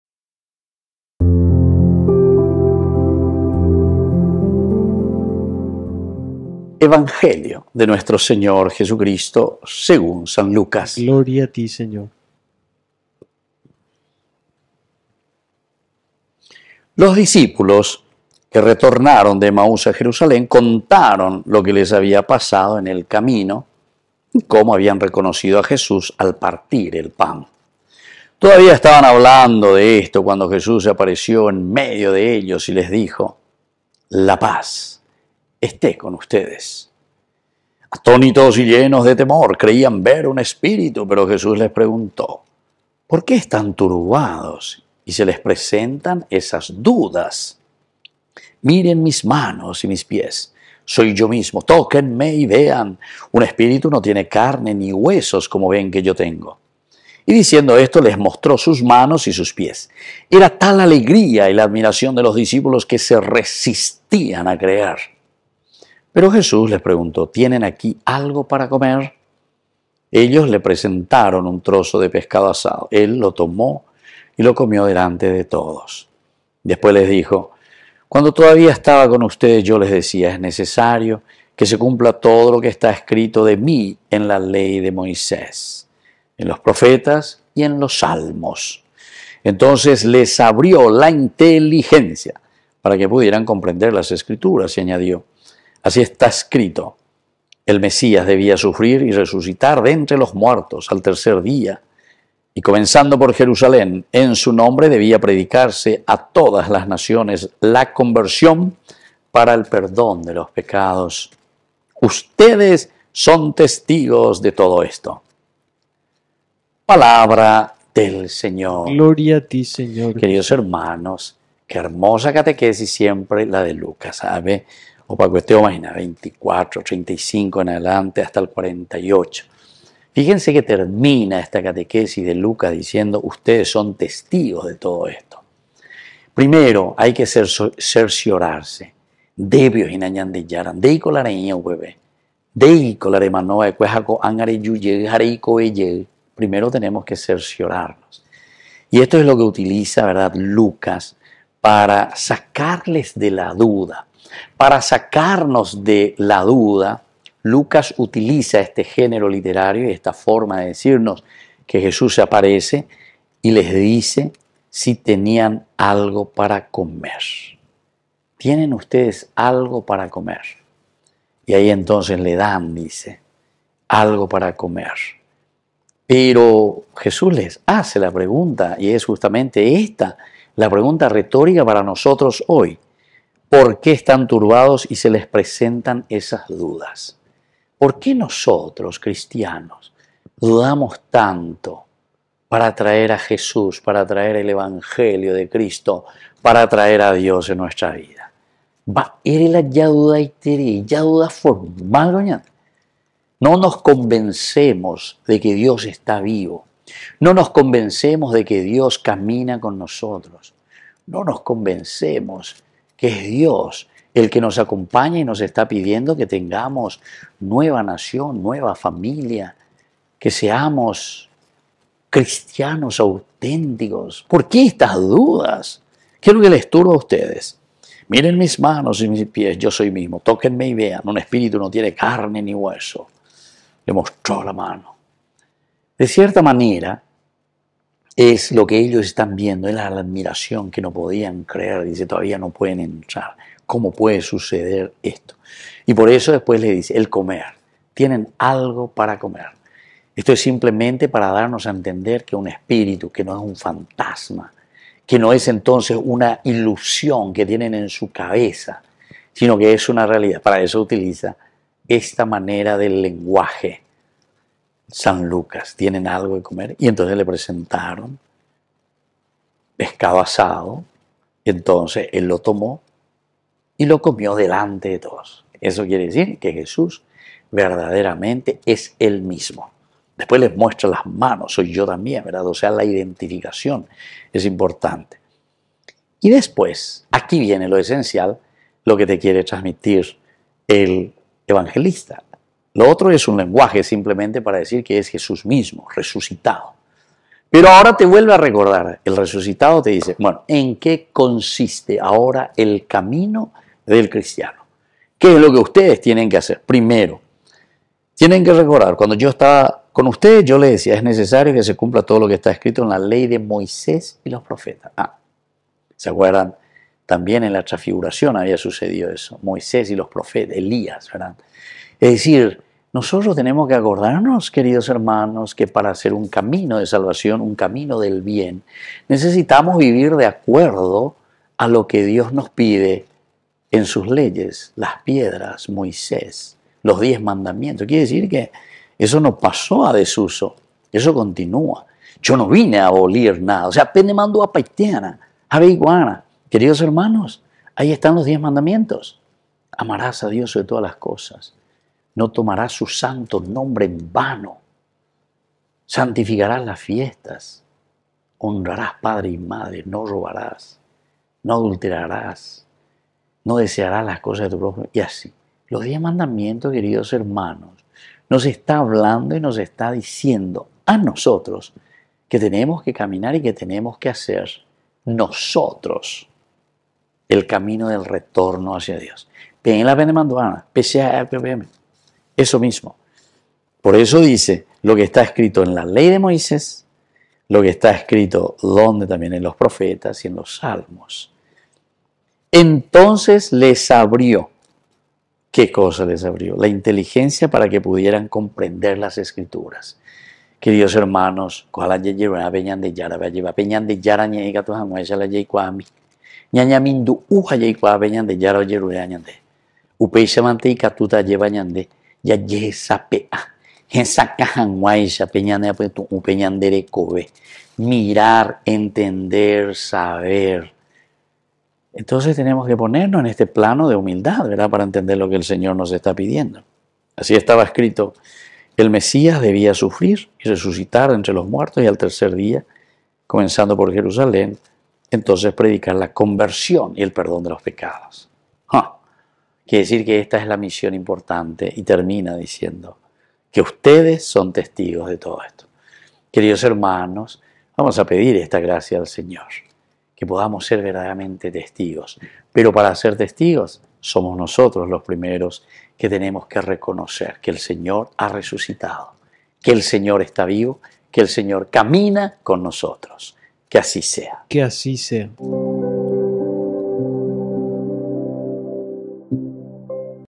HOMILIA JUEVES 13 DE ABRIL DEL 2023
Lucas describe el encuentro del Resucitado con sus discípulos